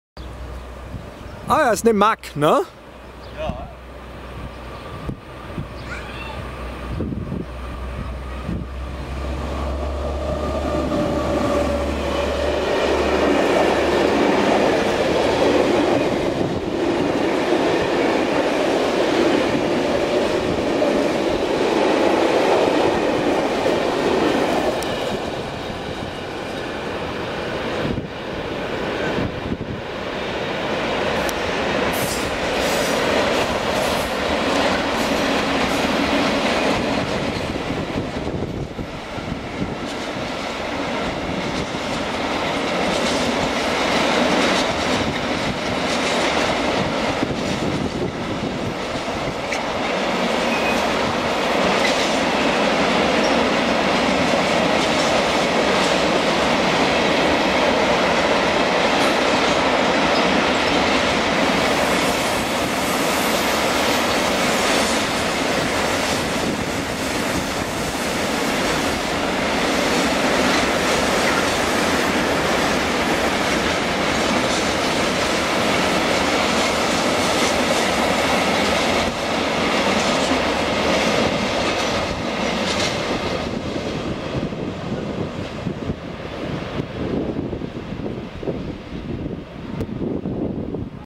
Trainspot: MaK mit Müllzug kreuzt Güterzug
Rückfahrt von der Müllverbrennungsanlage auf der Friesenheimer Insel in Mannheim. Bespannt mit der AVG 469, einer MaK G 1206, ist der Leerzug in Richtung Karlsruhe unterwegs. Entgegen kommt eine BR185 Railion vor einem langen Güterzug in Richtung Mannheim Güterbahnhof.